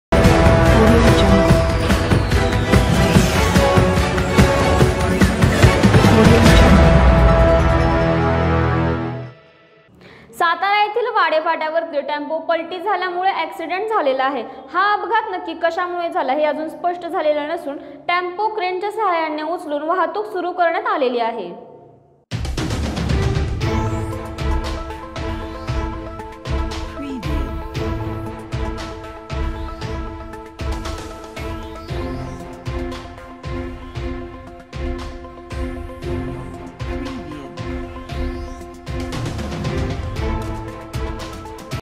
सातारा वाढे फाटा येथे रस्त्यावर टॅम्पो पलटी : अपघाताचे कारण अस्पष्ट
सातारा न्यूज